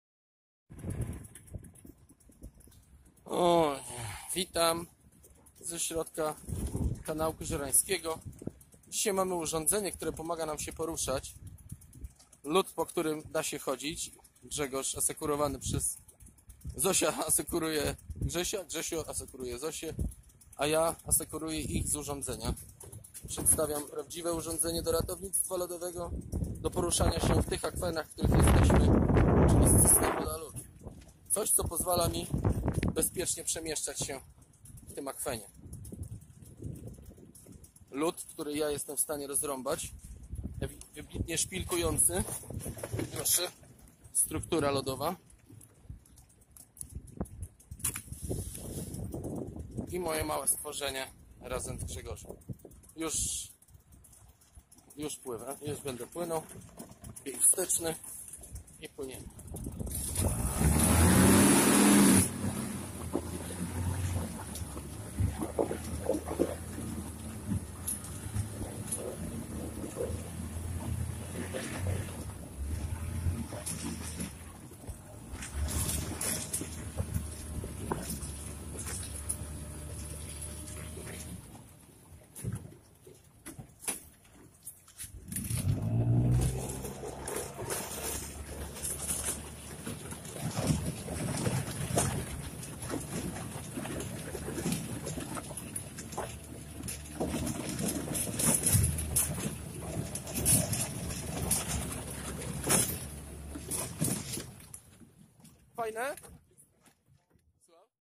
Kruchy lód i system woda lód
W środowisku lodu roztopowego należy mieć wsparcie w postaci urządzenia pływającego. Sanie lodowe typu system woda lód są w tych warunkach narzędziem doskonałym.